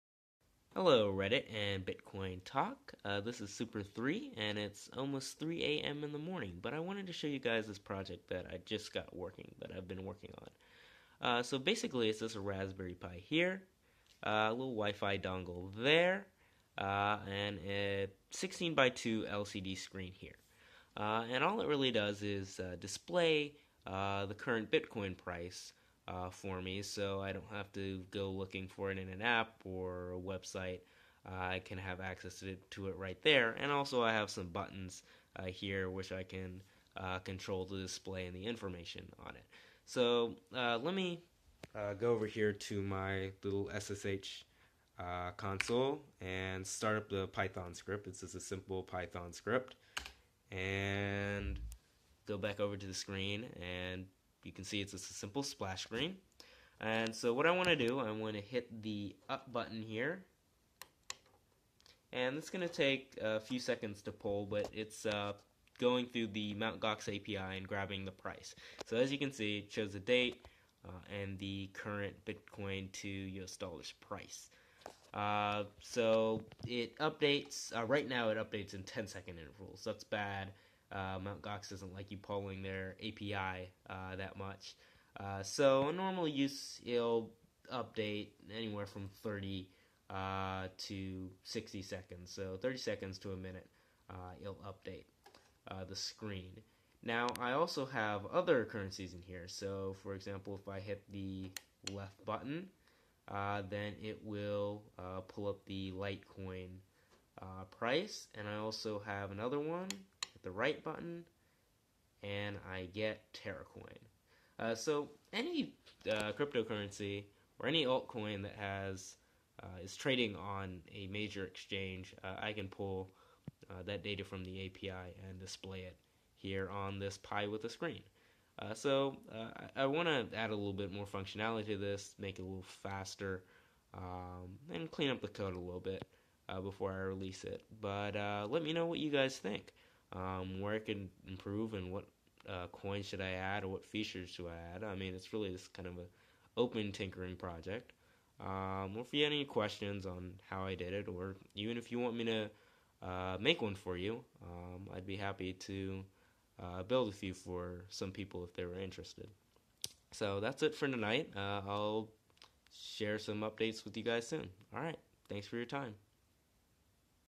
Raspberry Pi (Bitcoin, Litecoin, Terracoin) LCD Price Ticker
Just a little late night project. If you want more info on the project see the BitcoinTalk post: